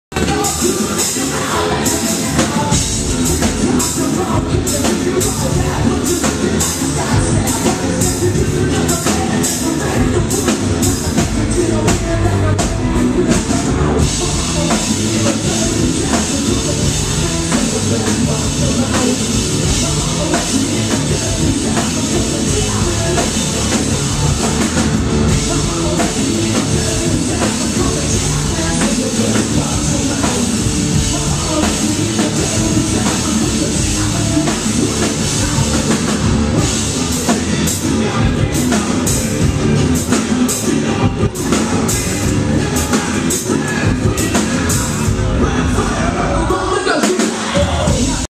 Back stage at the Family Force 5 show at Camp Electric
These guys were nuts! Heck of a stage presence!